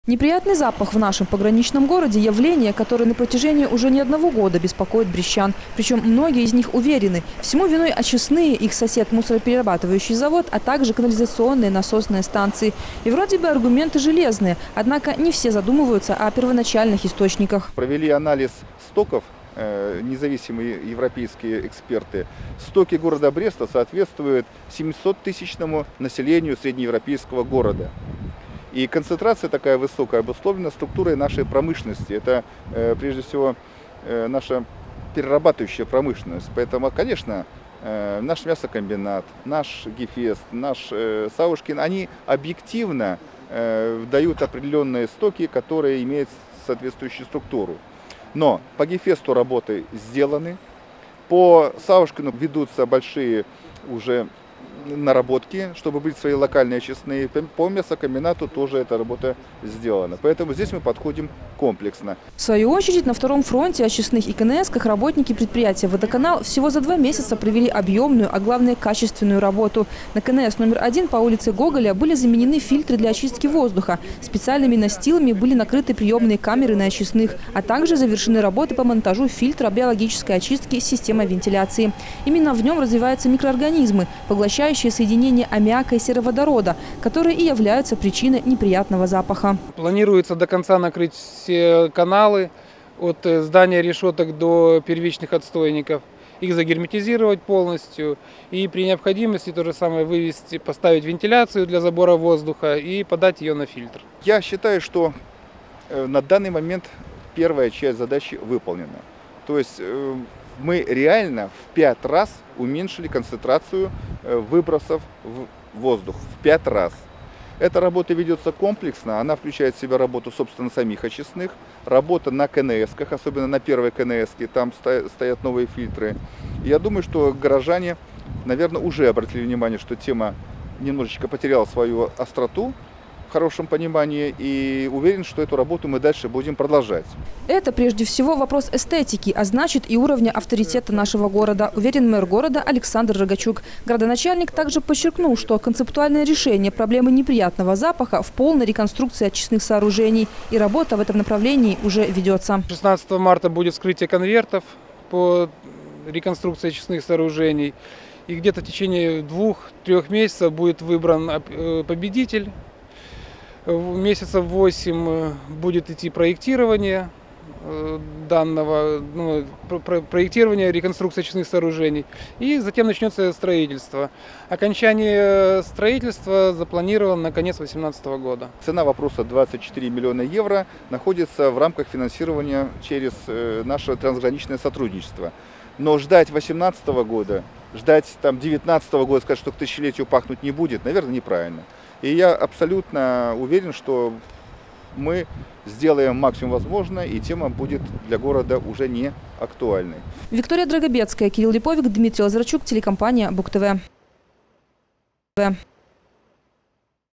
2016-03-04 г. Брест. Проблема неприятного запаха в городе. Телекомпания Буг-ТВ.
Проблема неприятного запаха в Бресте: мэр города дал оценку работе сотрудников Брестводоканала по устранению проблемы.
ПОДВ.: К другим темам. Очередной шаг в решении проблемы неприятного запаха предпринят в городе над Бугом. Напомним, в декабре 2015 года в Брестском горисполкоме состоялось совещание с участием представителей коммунальных служб и ведомств, ответственных за экологическую ситуацию в областном центре. Его целью был поиск и разработка мер по устранению неприятного запаха, источники которого - городские очистные сооружения, канализационно-насосные станции, а также мусороперерабатывающий завод.
Спустя два месяца, 3 марта мэр города Александр Рогачук проверил выполнение задач, данных на совещании, посетив комплекс очистных сооружений Бреста.